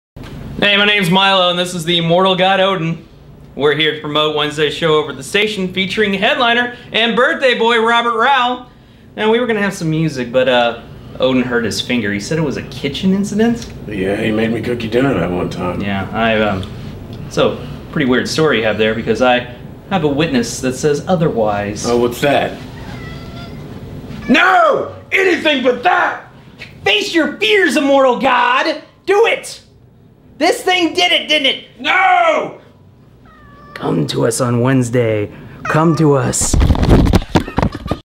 Milo and Odin - Delete Comedy Promo 2
Odin can conquer many things, except his fear of cats.

Plus, check out Delete Comedy Open mic every Wednesday at 8:30.  This week, we have Milo and Odin opening for Robert Rau